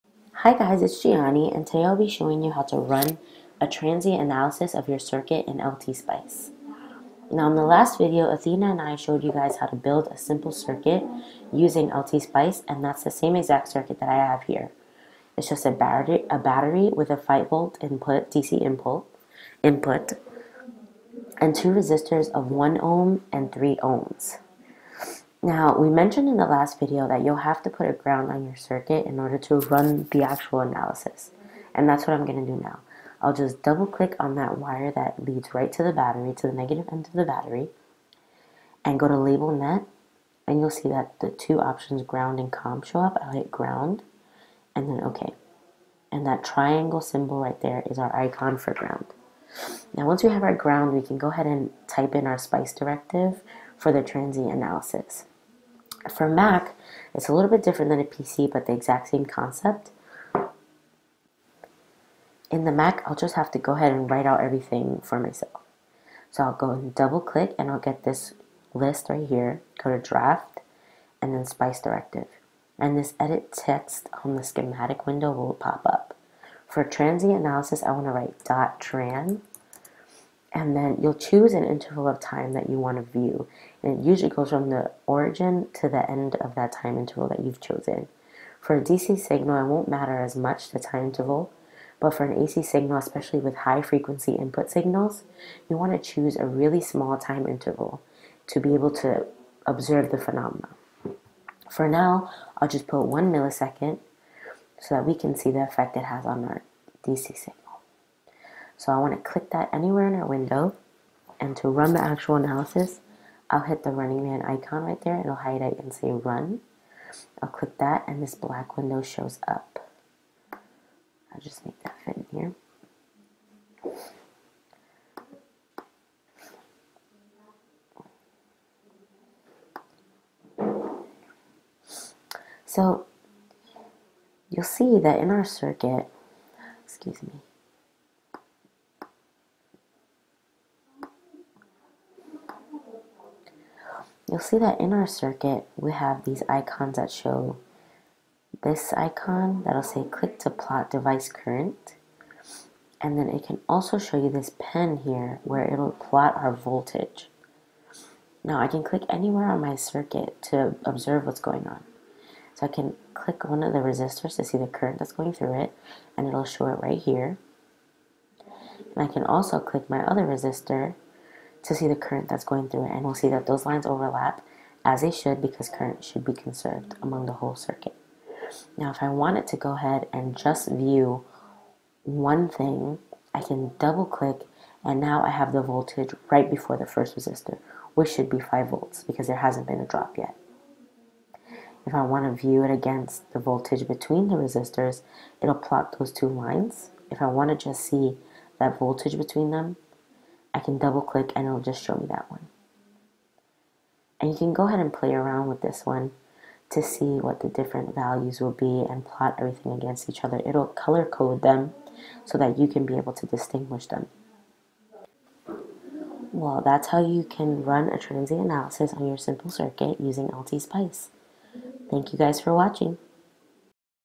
Transient Analysis in LTSpice
How to run a transient analysis of a simple circuit using LTSpice